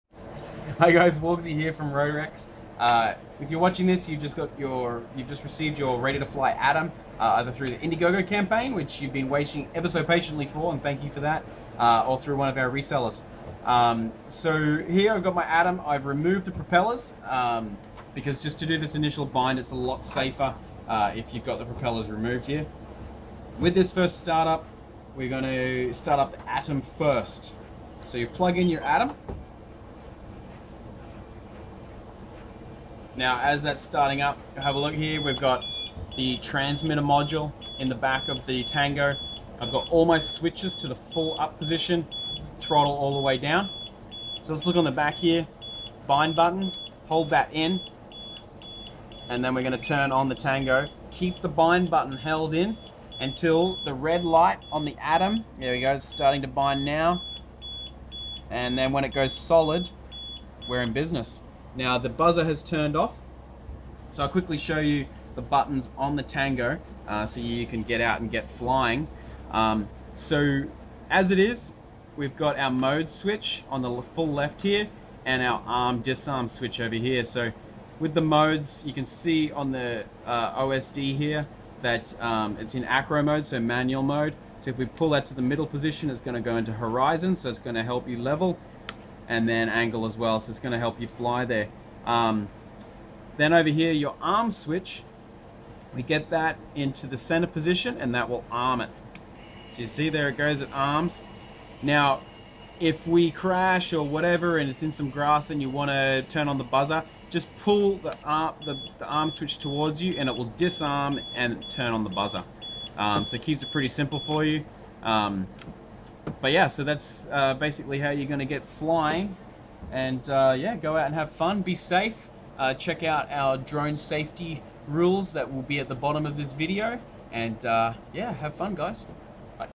Atom V2 RTF - Getting Started
Quick explanation to get your 'Ready to Fly' Atom V2 in the air safely.